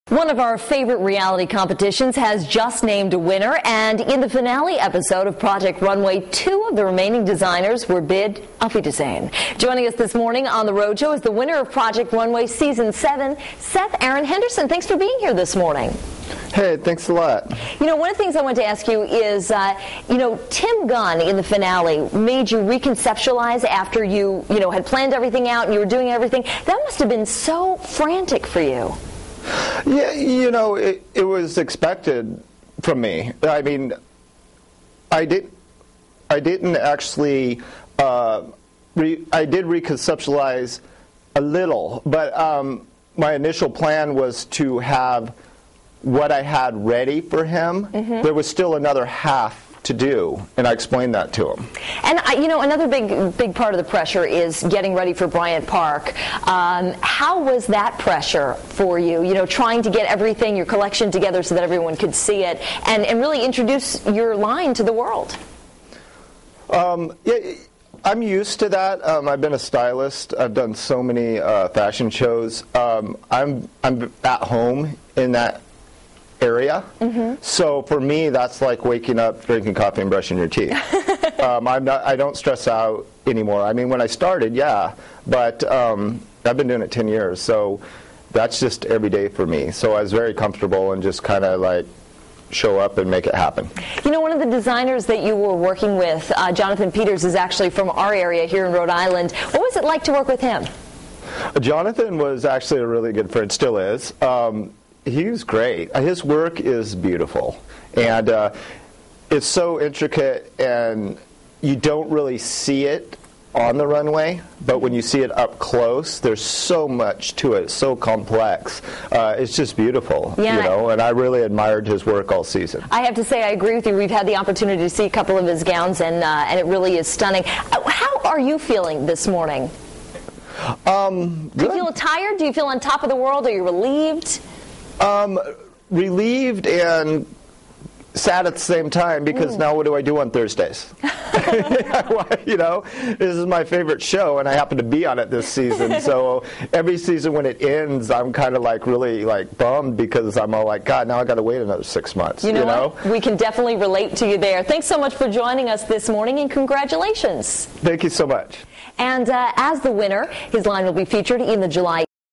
Interview: Seth Aaron Henderson
After showing a 1940s-German-and-Russian- military-inspired 10-piece collection at Bryant Park during Fashion Week in February, Northwest-based designer Seth Aaron Henderson has been named the winner of Season 7 of "Project Runway" Beating out fellow designers Emilio Sosa and Mila Hermanovski, Henderson was praised for his cohesive, beautifully constructed, show-stopping collection by the show's judges. He walked off with an editorial feature in Marie Claire magazines July issue, and and a cash prize of $100,000 from LOréal Paris to launch his own fashion line, among other prizes.